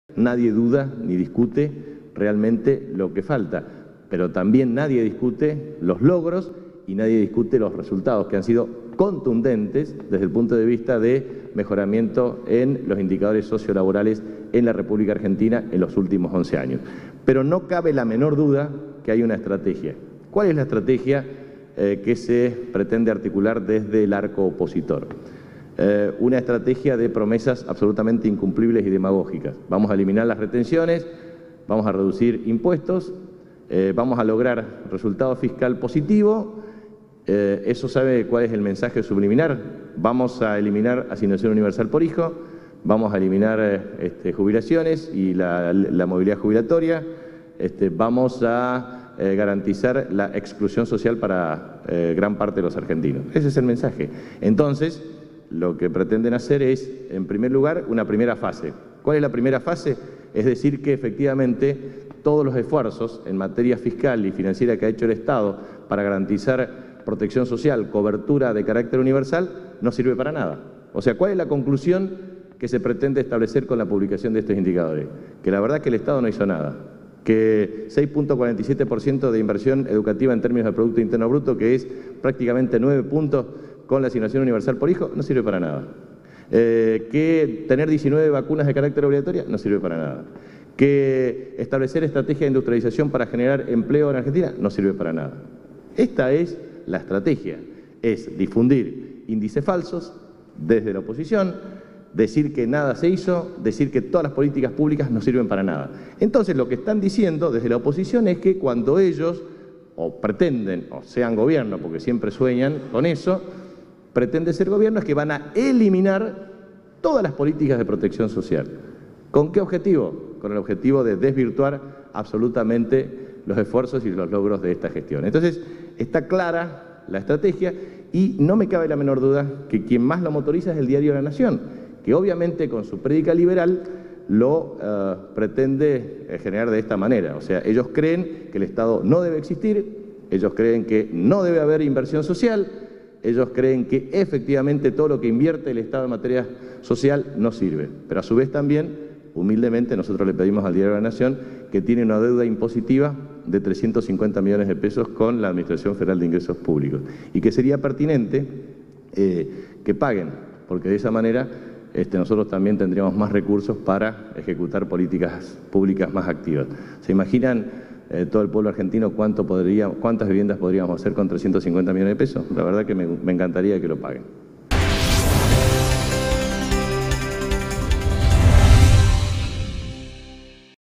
Capitanich cuestionó estrategia de oposición para "desvirtuar" acción del gobierno
Capitanich cuestionó estrategia de oposición para "desvirtuar" acción del gobierno y criticó al diario La Nación

El jefe de Gabinete, Jorge Capitanich, aseguró hoy que existe una estrategia impulsada desde la oposición para hacer creer que “el Estado no sirve”, y “desvirtuar” los esfuerzos realizados por el gobierno en materia de inclusión social, lo que se refleja por ejemplo en el informe elaborado por el observatorio de la Universidad Católica Argentina (UCA), que publica hoy el diario La Nación.

“Hay una estrategia desde el arco opositor, que se basa en promesas incumplibles y demagógicas, y en desvirtuar todos los esfuerzos realizados por el Estado en materia fiscal y financiera para protección social, con el propósito de mostrar que no sirven para nada, y que el Estado no sirve”, dijo Capitanich en su habitual conferencia de prensa matutina en Casa de Gobierno.

Agregó que por eso desde la oposición difunden “índices falsos” que incluso se contradicen con los informes de organismos internacionales, como el Banco Mundial, que destaca la política pública y sus resultados en Argentina y Uruguay, y específicamente en Argentina la reducción abrupta de los niveles de pobreza.

También recordó el informe de la FAO (Organización de las Naciones Unidas para la Alimentación y la Agricultura), que dijo que prácticamente se había erradicado el hambre e indigencia en Argentina, de la ONU a través del PNUD(Programa de las Naciones Unidas para el Desarrollo), que colocó a la Argentina entre los 49 países de muy alto desarrollo humano, y la CEPAL (Comisión Económica para América Latina y el Caribe), que destacó el resultado de las políticas activas en materia de desarrollo económico e inclusión.

El jefe de ministros aseguró que la primera fase de la estrategia opositora es mostrar que lo que se hizo no sirve, pero advirtió que lo que sigue en realidad es “la eliminación de todas las políticas de protección social para desvirtuar los logros y esfuerzos de esta gestión”.

Incluso Capitanich señaló que el diario La Nación es uno de los que más trabajan en este sentido y que detrás de esta actitud subyace su convicción de que “el Estado no debe existir, no debe haber inversión social y que lo que invierte el Estado no sirve”.

“Primero el diario La Nación tendría que cumplir con su deuda impositiva, de 350 millones de pesos, así tendríamos más recursos para políticas activas”, manifestó el funcionario.

Finalmente, dejó claro que “la oposición inventa números e institutos en apariencia seria que en realidad oculta su ideología”, e invitó al observatorio de la UCA a “tener un debate profundo” con el gobierno, que incluya el debate de la metodología con la que se elaboraron los resultados.